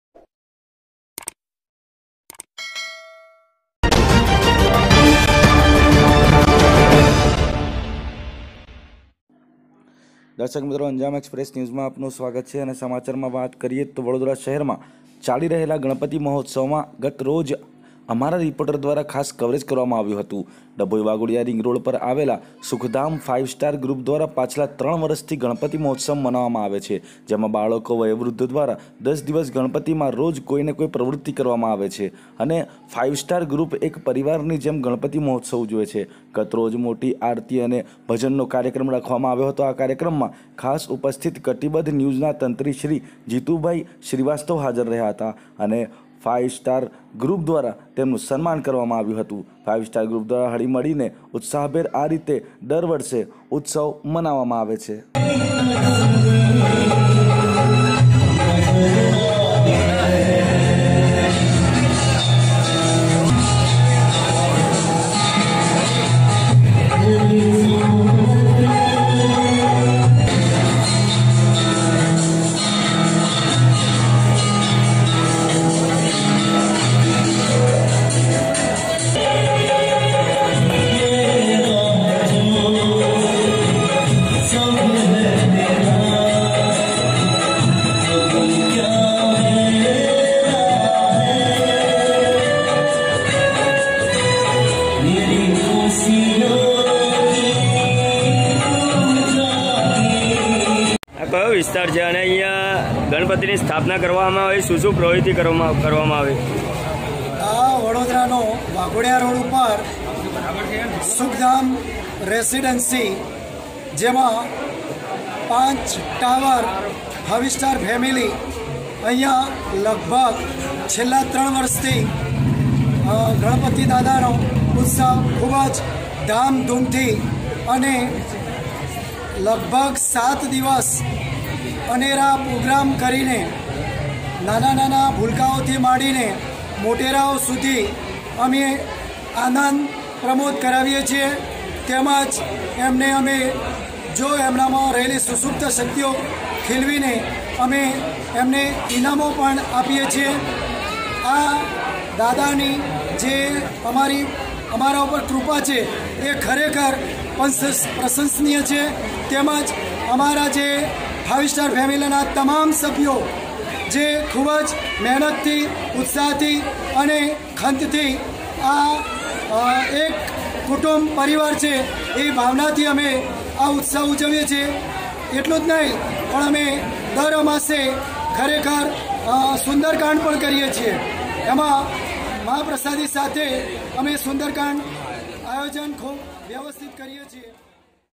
Dabhoi - સુખધામ ફાઈવ સ્ટાર ગ્રુપ દ્વારા પાછલા ત્રણ વર્ષથી ગણપતિ મહોત્સવ મનાવવામાં આવે છે.
જે કોઈ ને Social Media News Group માં એડ થવું હોય એ આપેલ નમ્બર પર પોતાનો પરિચય અવશ્ય આપી એડ થઈ શકે છે - A.R.Patel - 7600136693 (Whatsapp)